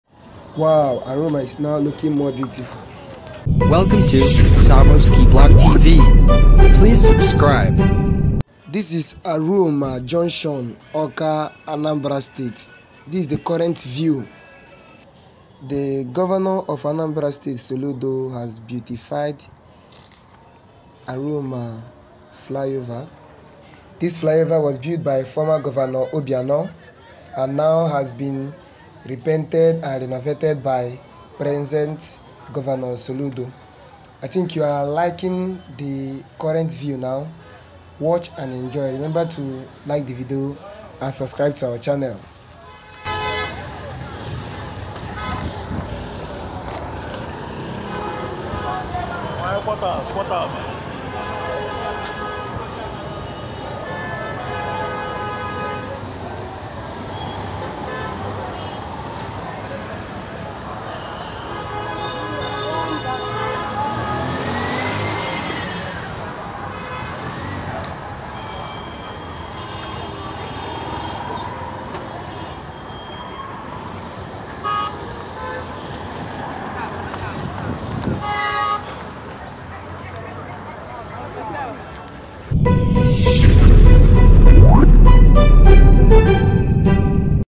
ARROMA FLYOVER NEW LOOK || AWKA ANAMBRA STATE
This is current view of Arroma Awka Anambra State Flyover, built by former Governor Willie Obiano and presently renovated by Present Governor, Soludo.